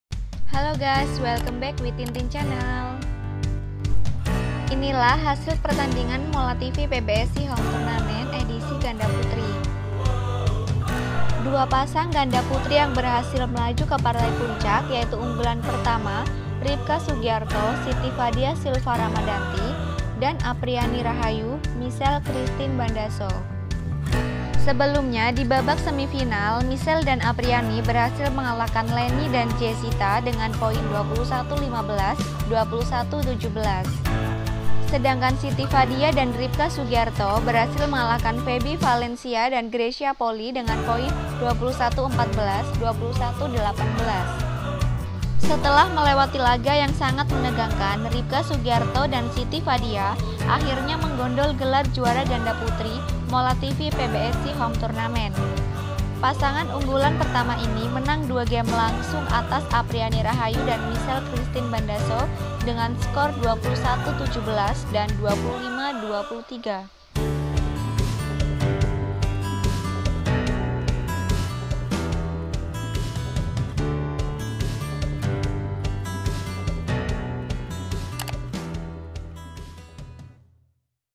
POINT SENGIT!! RIBKA SUGIARTO/SITI FADIA SILVA JUARA MOLA TV PBSI HOME TOURNAMENT || womens double
RIBKA SUGIARTO/SITI FADIA SILVA JUARA MOLA TV PBSI HOME TOURNAMENT
Setelah melewati laga yang sangat menegangkan, Ribka Sugiarto/Siti Fadia Silva Ramadhanti akhirnya menggondol gelar juara ganda putri Mola TV PBSI Home Tournament. Pasangan unggulan pertama ini menang dua game langsung atas Apriyani Rahayu/Mychelle Crhystine Bandaso dengan skor 21-17, 25-23.
Apriyani Rahayu/Mychelle Crhystine Bandaso tak dapat mencuri satu game pun dari Ribka Sugiarto/Siti Fadia Silva Ramadhanti di babak final ganda putri PBSI Home Tournament. Meskipun demikian, Apriyani/Mychelle memberikan perlawanan sengit yang sangat menarik untuk disaksikan.